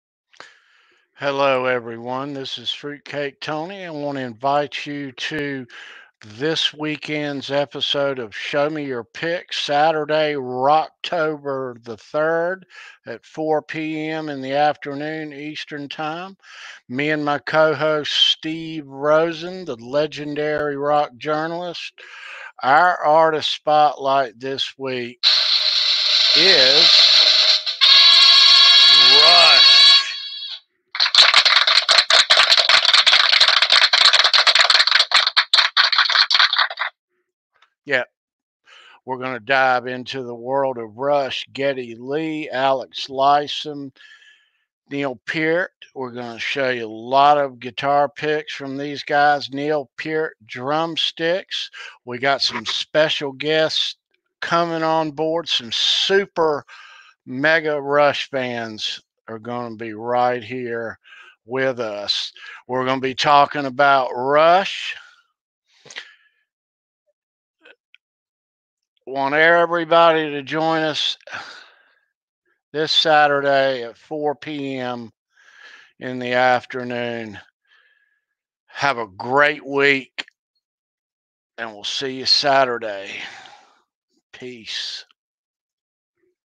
SHOW ME YOUR PICK #16 RUSH *promo*
SHOW ME YOUR PICK #16 RUSH with Fruitcake Tony, Legendary Rock Journalist Steve Rosen & Special Suprise Guests. This Saturday Roctober 3, 2020 at 4PM ET.
*will be giving away a Rush Alex Lifeson 2008 Tour Guitar Pick*